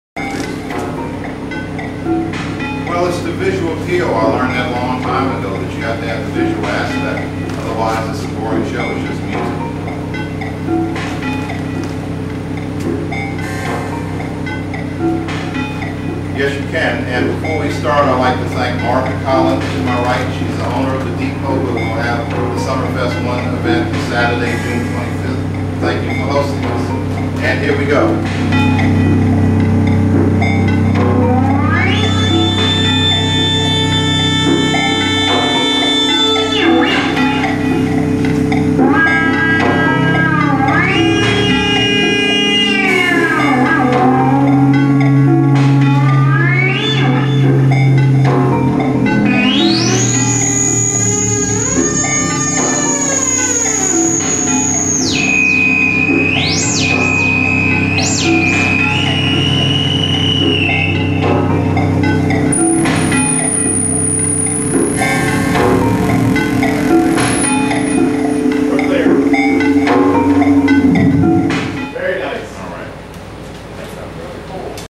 baltimore sdiy group fox45 tv promo #2,6-20-2011.wmv
here is a back-of-the-room look at tv promo #2 done by members of the baltimore sdiy group, logan mitchell sr. and david vosh, promoting the groups  "summerfest 2011" event being held at  "the depot"  in baltimore, md. on june 25, 2011 and july 23, 2011. the tv promos were carried by baltimores fox45 as features presented during their morning show. this one aired around 7:10 on 6/20/2011